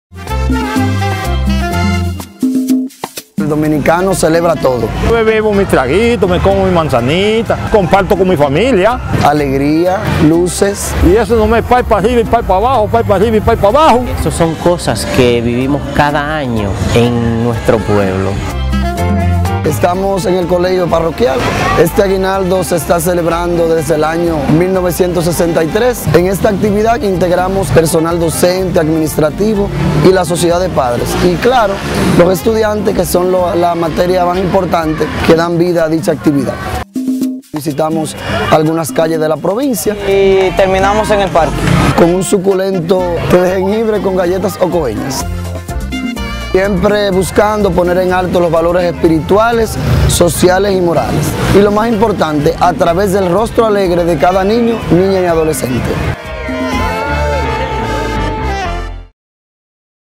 Alegre Navidad - Aguinaldo Ocoa
San José de Ocoa es pura fiesta desde que llega Navidad, pero no cualquier fiesta.

Ocoa mantiene una tradición desde hace 56 años que refleja muy bien el espíritu festivo de su gente.